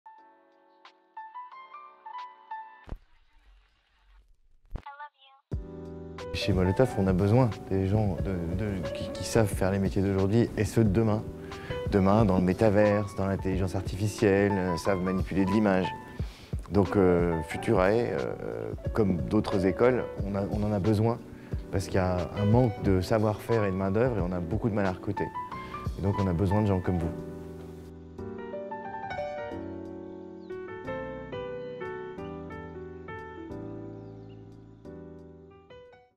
ConférenceCEO - Mot de Jean-David BLANC Président Directeur Général de Molotov.
Découvrez le témoignage de Mr. Jean-David Blanc, dPrésident Directeur Général de  Molotov, suite à sa conférence à l'école Futurae.